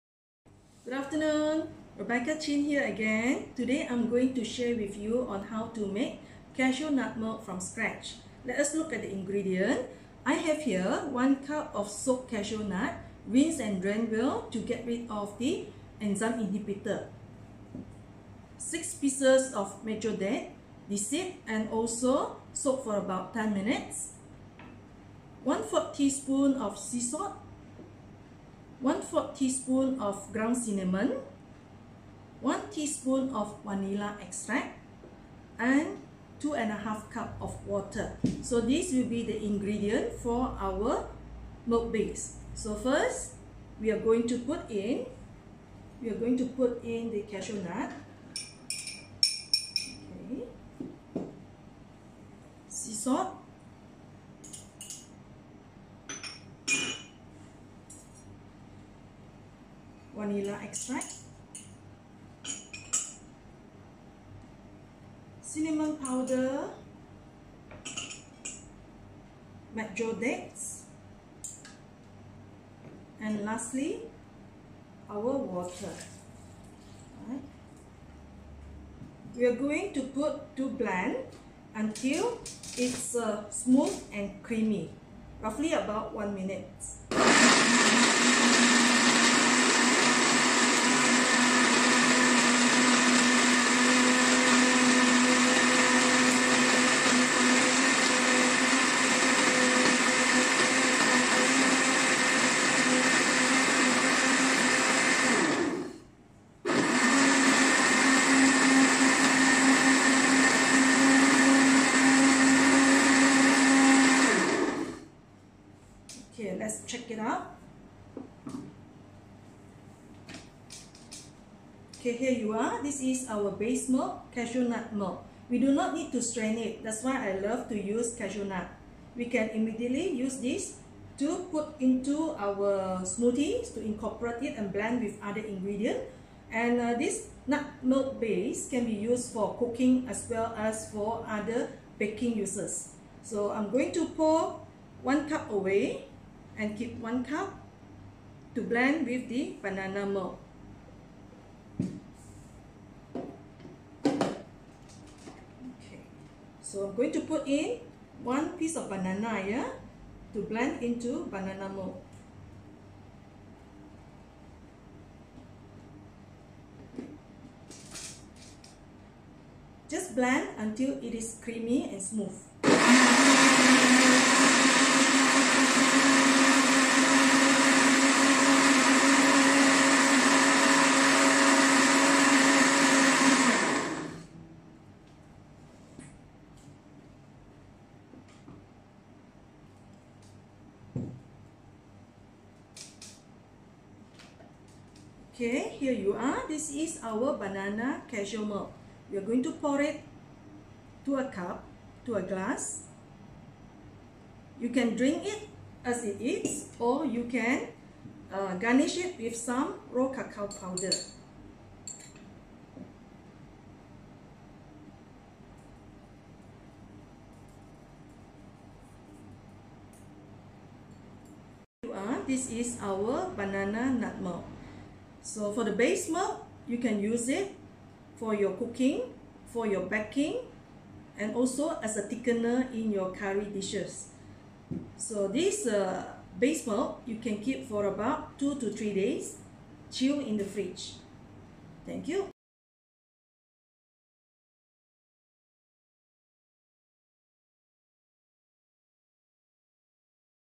How to make Cashew Banana Milk Smoothie
Hi. I'm Rebecca Chin, a Culinary Nutrition Practitioner. In this video, I'm sharing with you on how to make Cashew milk from scratch and to incorporate it to make Cashew Banana Milk Smoothie.